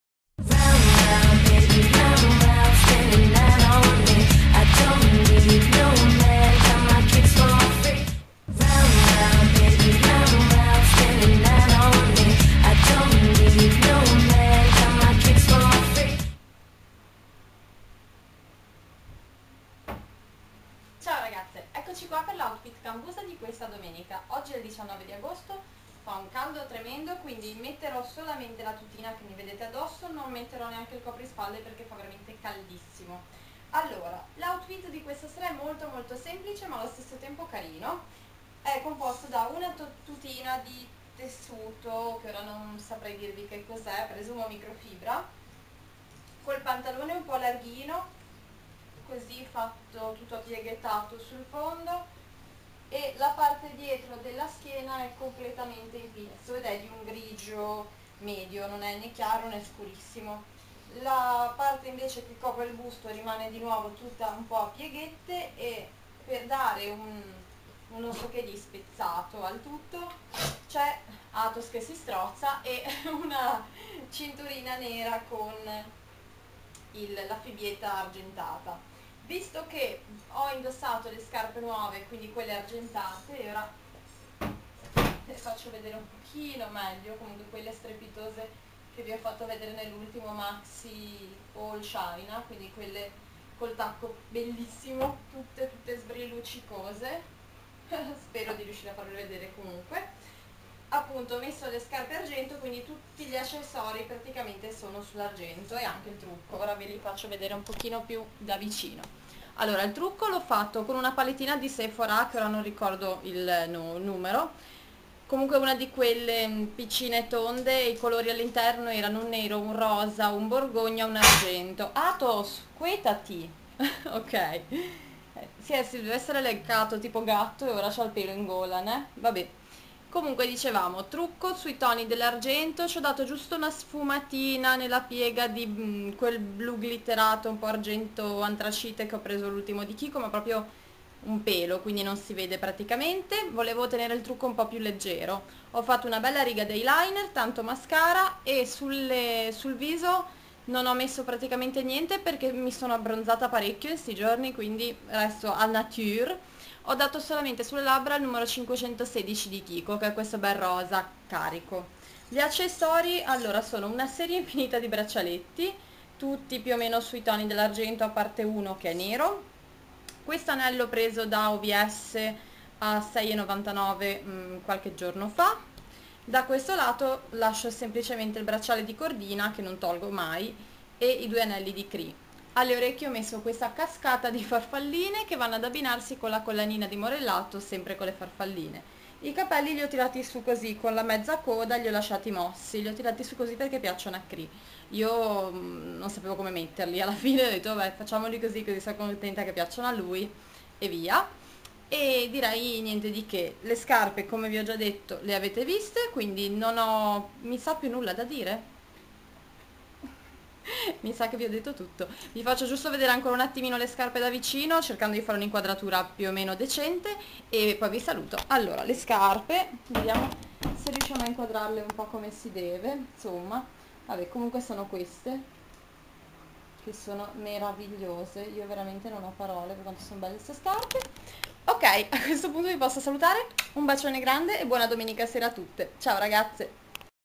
Momo video O O T N GRIGIO ARGENTO E TACCHI ALTI
ecco l outfit da sera che avevo ieri x andare in cambusa .. spero vi piaccia